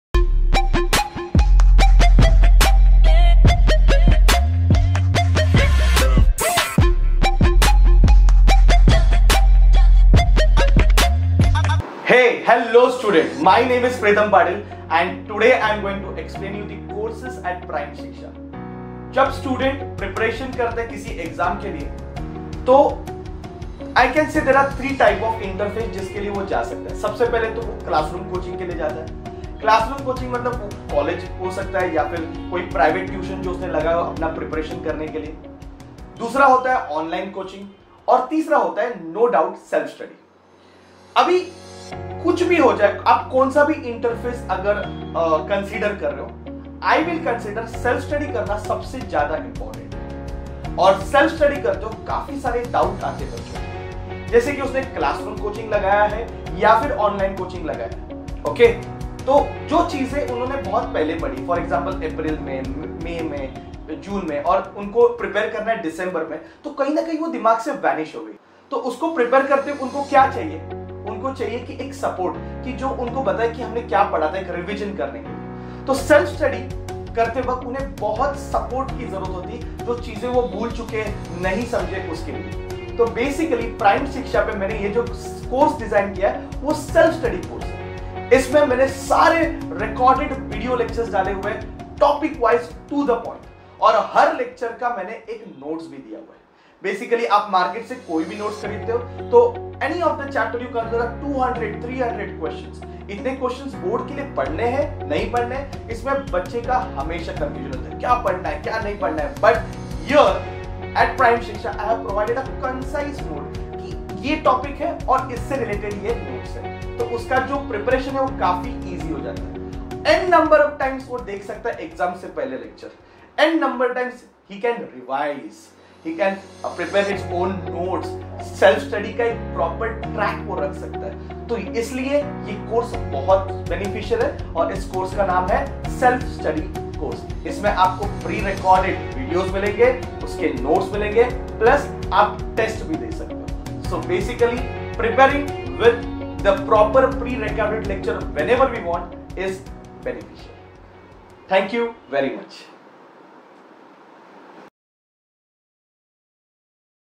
WHAT ARE THE COURSES AT PRIME SHIKSHA BY PREETAM PATIL (PPT)
This video is advertise base content in which you will get to know about how you can learn physics in the easiest way by pre recorded videos on primeshiksha.com by PREETAM PATIL (ppt)